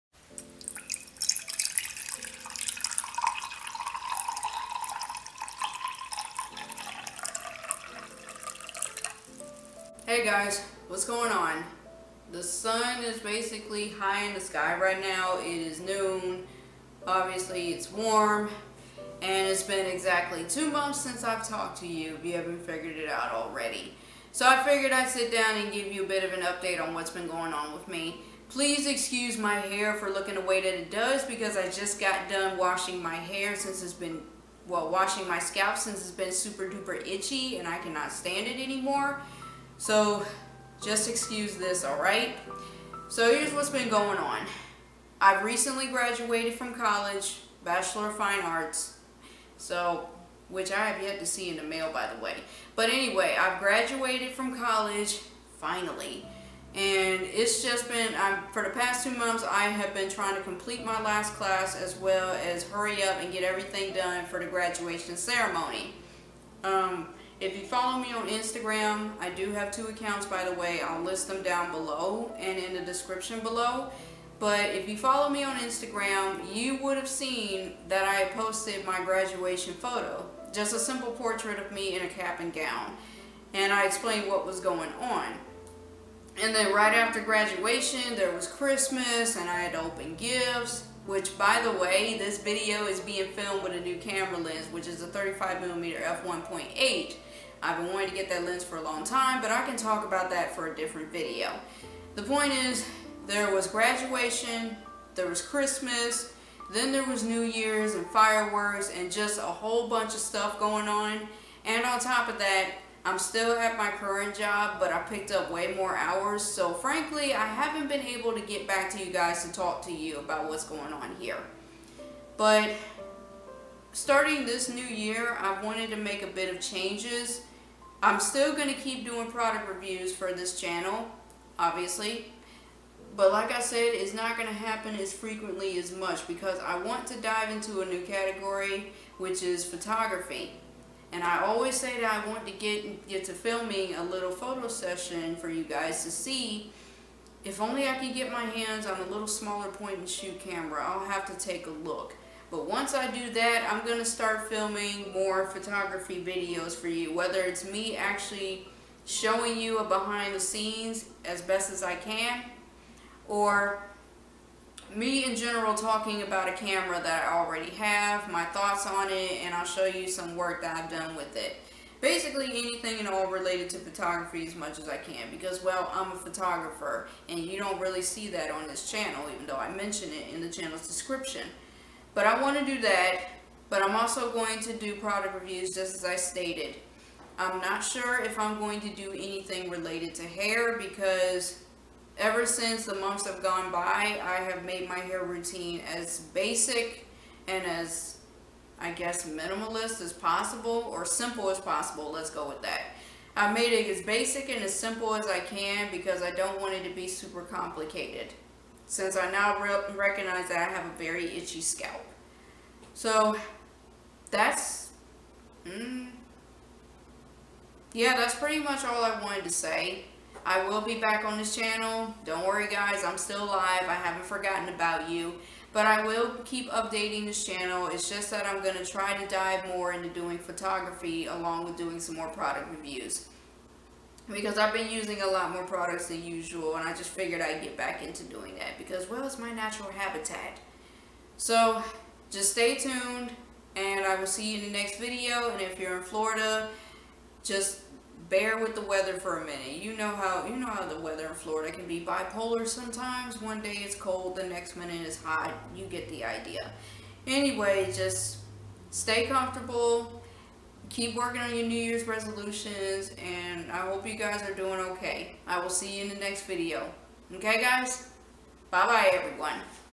Life Update | College Graduation and Channel's Future
It's been two and a half months since I've talked to you...and I have a lot to say within a short period of time.

But I'm back and nothing bad has happened to me, so don't worry.

So...thanks to everyone for their patience and support. I appreciate it greatly.

Instagram Accounts - @theartfulflowerblog and/or @jruffinphoto